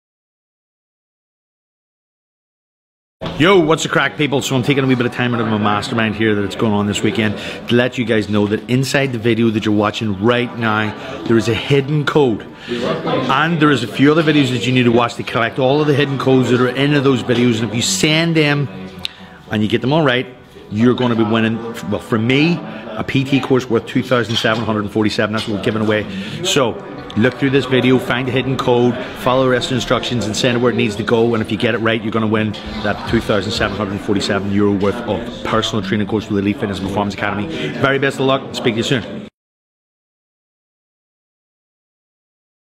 The Big 4K Giveaway
• Elite Fitness & Performance Academy Coaching Certification LVL3 & LVL4 part time course in Dublin (worth €2747)
• GRIP Gym Gear custom printed workout bench
• Gearbag accessory hamper: Hickeis laces, Picsil Grips, HSPU Mat, RX Jump Rope, Rocktape & Tshirt.
• Pair of Tough Mudder Ireland entry tickets for July.
• FitFodder, 56 piece premium protein bundle.
• OLY Clothing Green Jumper & Baseball top.
• Atlantic Barbell Gym Equipment weighted vest.
• Celtic Series: 9 qualifier entries, VIP for your gym, 9 tshirts or snapback caps at the Finals on June 23rd.

The first person to send all 7 hidden codes from each video across the following Facebook pages. Will win !.
1. Elite Fitness & Performance Academy
2. GRIP Gym Gear
3. Gearbag
4. FitFodder
5. OLY Clothing
6. Atlantic Barbell Gym Equipment
7. Celtic Series Fitness Racing